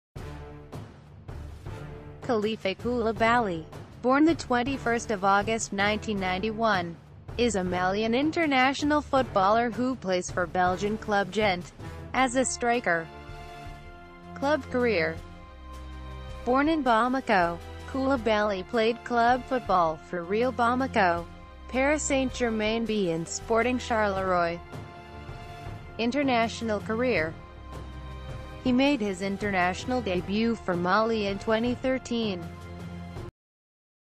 Kalifa Coulibaly Documentary
Kalifa Coulibaly (born 21 August 1991) is a Malian international footballer who plays for Belgian club Gent, as a striker.